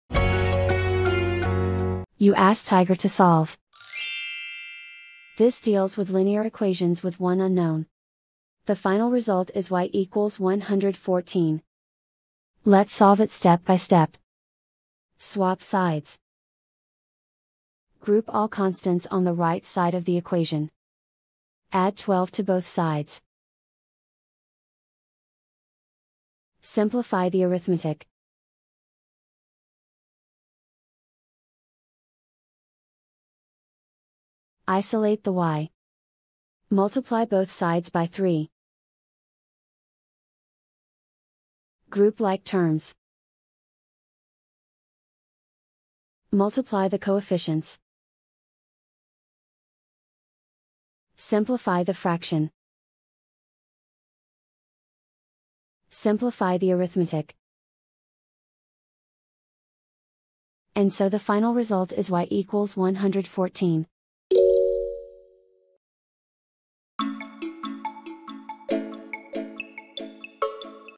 Solve 26=y/3-12: Linear Equation Video Solution | Tiger Algebra
Struggling with 26=y/3-12? Watch our step-by-step video on Tiger Algebra to master this linear equation effortlessly.